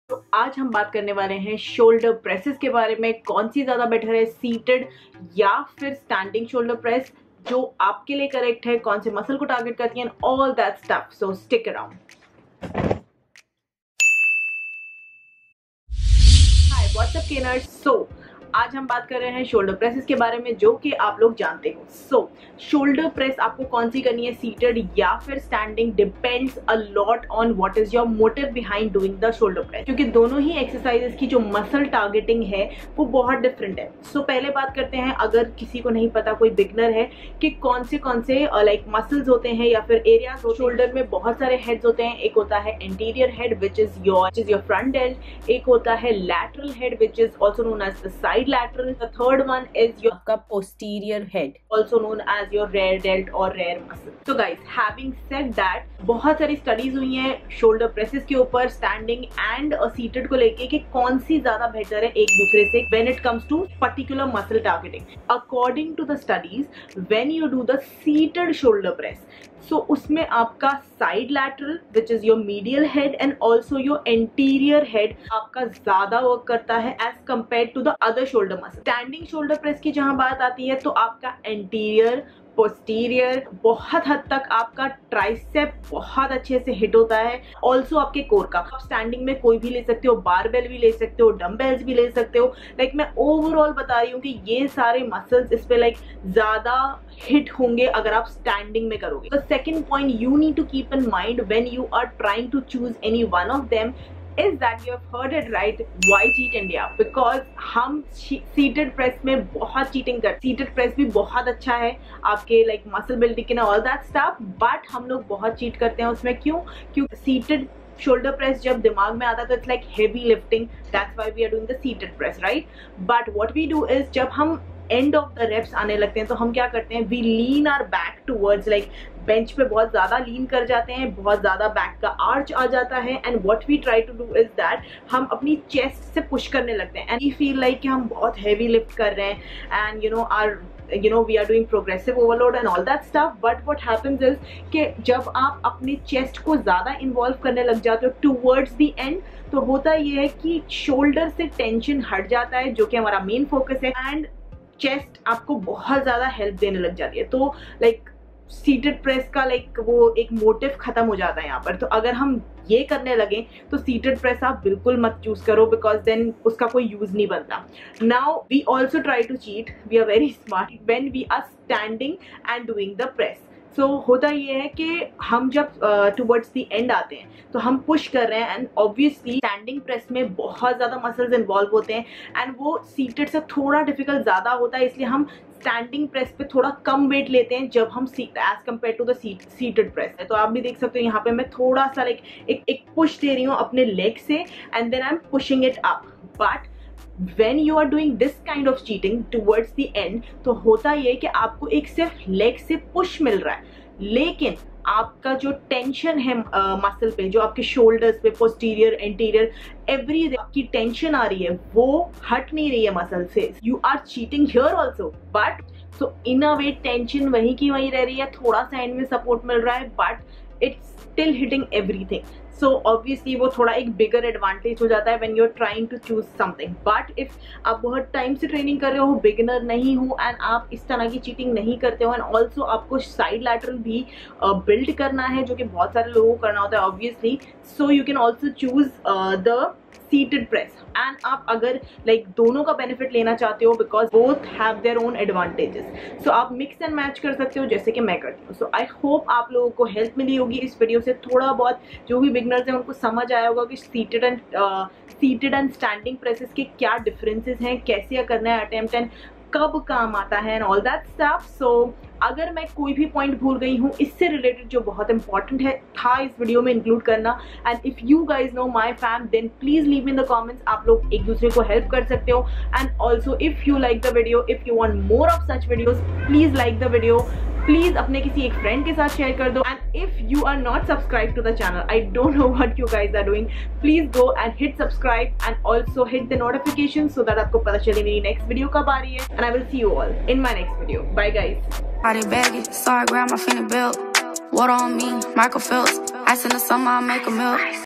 Seated Shoulder press V/S Standing Shoulder press in HINDI- (HIT ALL HEADS WITH THIS INFORMATION!!!)
This video is about shoulder presses. There are two kinds of shoulder presses - Seated & Standing, both have different advantages and disadvantages, both hit different parts of the shoulder head.

In this video, you will learn how to do shoulder presses effectively and also the difference between the two will help you choose the best one for yourself.

SOCIAL MEDIA!!

Makeup

KANZAH SYED